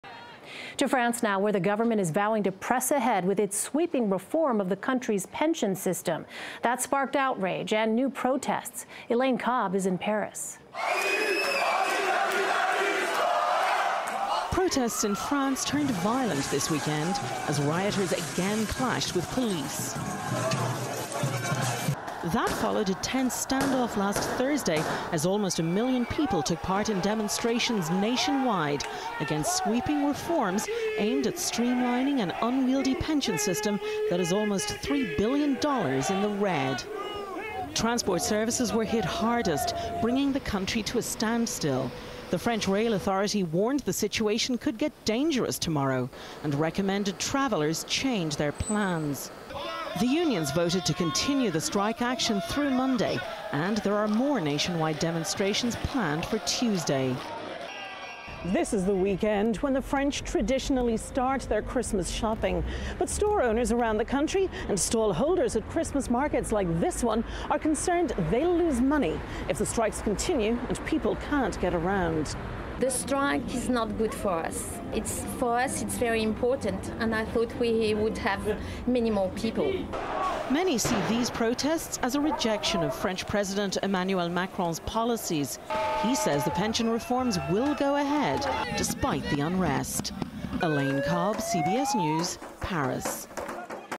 France government vowing to press ahead with proposed reform
The French government and President Emmanuel Macron is vowing to press ahead with its sweeping reform of the country's pension system. This has sparked outrage and new protests across the country. Elaine Cobbe reports from Paris.